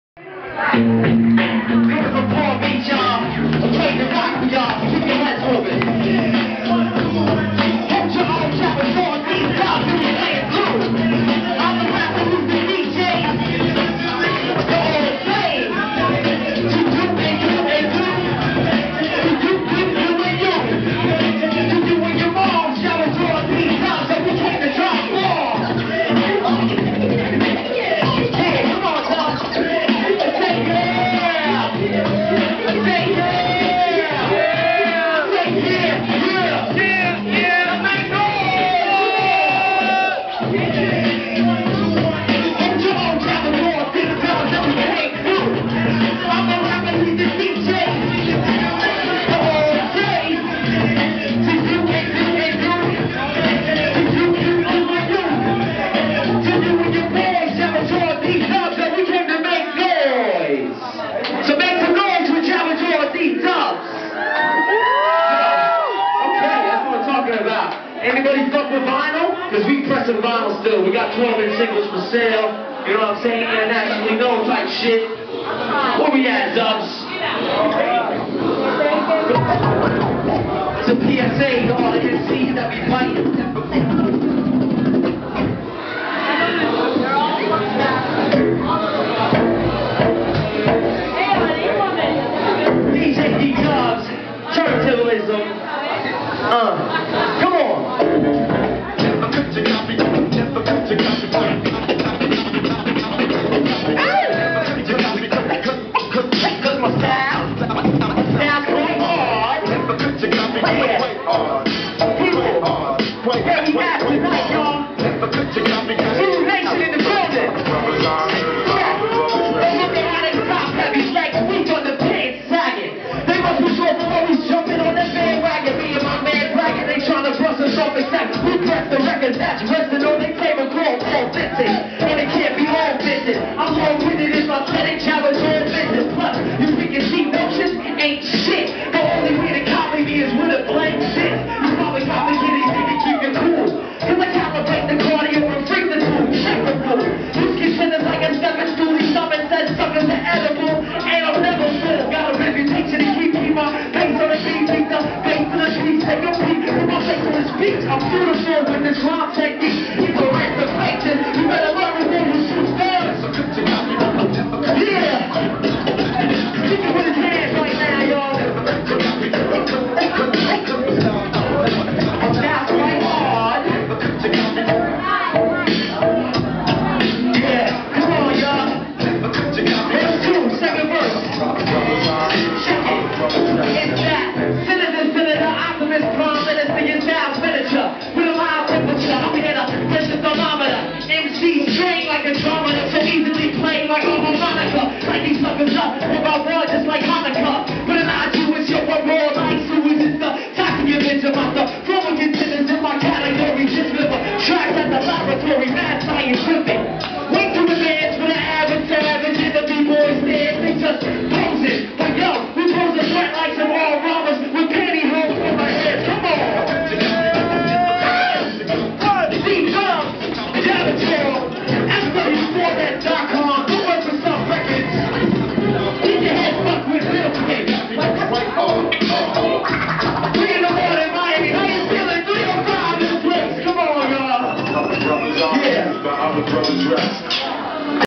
Footwork4self - Jabr Jaw & DJ Dee Dubbs - Zulu Arts Festival 2010
Jabr Jaw and DJ Dee Dubbs performing @ the Zulu Arts Festival - Loft 168 Downtown Miami - November 20th, 2010